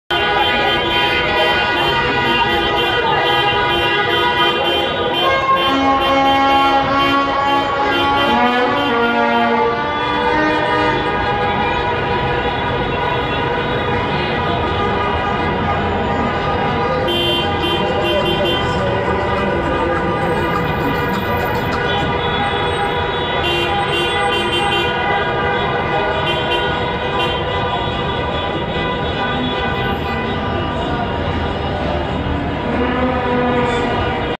FESTEJO DE ARGENTINA CAMPEÓN EN EL MARACANÁ
Así estuvieron las calles de Buenos Aires durante toda la noche. Ni que hablar del Obelisco. !!!